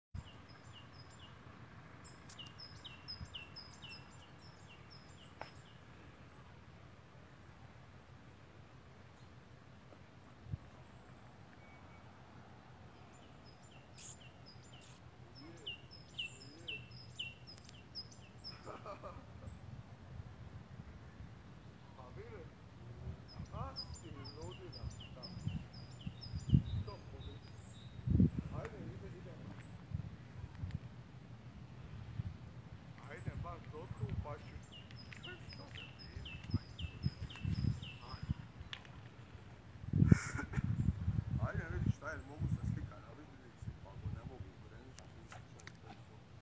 Zatvoren ulaz na Marjan
Policija je trakama obilježila ulaz na južnu stranu Marjana i na Prvu vidilicu i šetačima ljubazno preporučuje